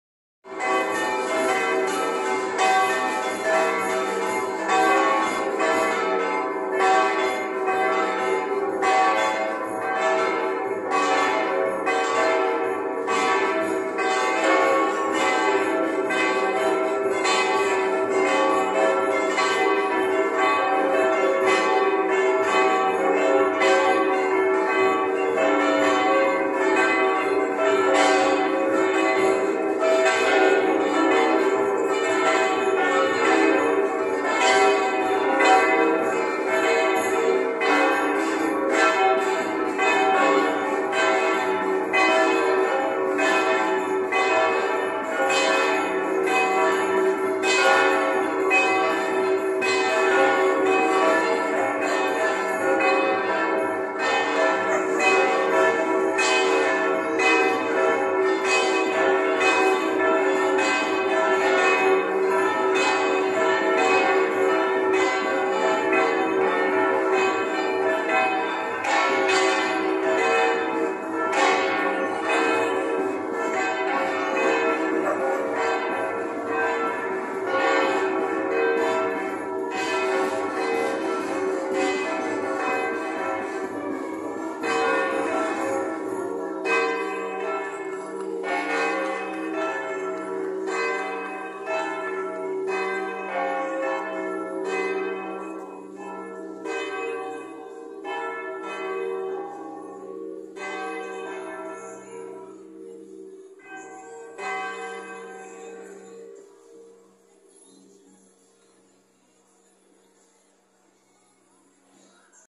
Plenum di Capodanno del "Campanone" - Pontremoli, 01.01.2024
Segno delle 17:30 per la S. Messa delle ore 18.
Video fatto di corsa e non bene. Cella campanaria illuminata, ma con diversi riflessi dovuti anche alla ripresa fatta con l'attuale cellulare. Alla fine, era meglio quello vecchio. ;-)